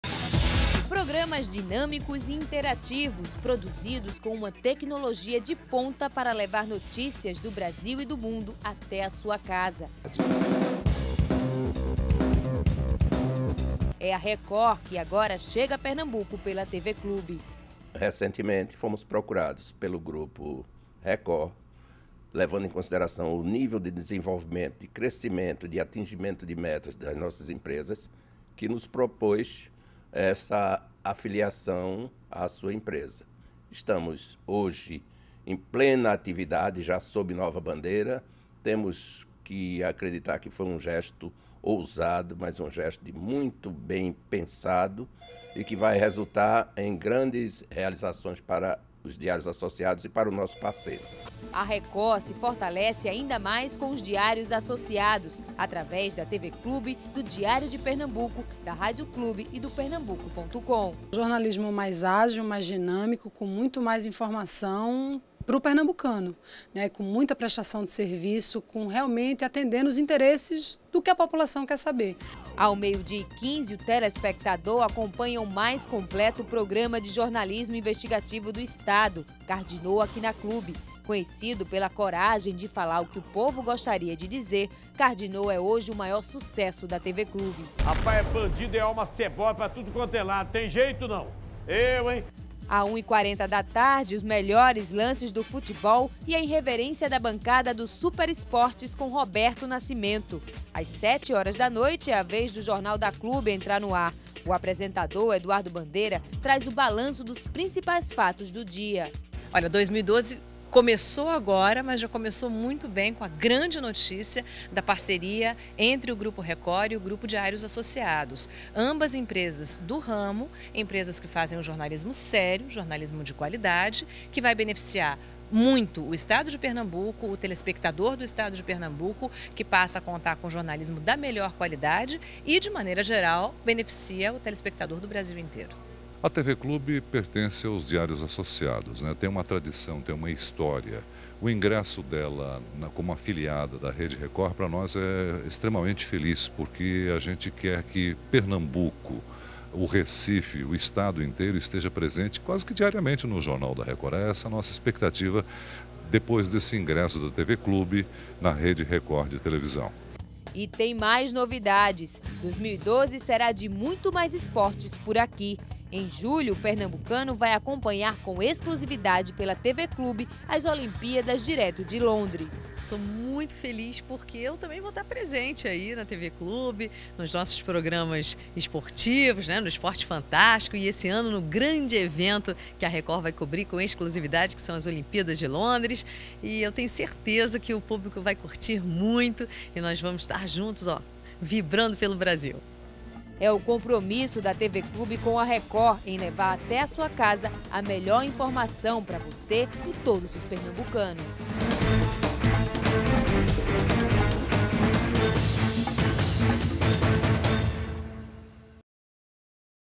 Veja como a parceria entre TV Clube e Record vai beneficiar os telespectadores pernambucanos
Parceria entre TV Clube e Record vai beneficiar os telespectadores pernambucanos com mais jornalismo de credibilidade e entretenimento de qualidade.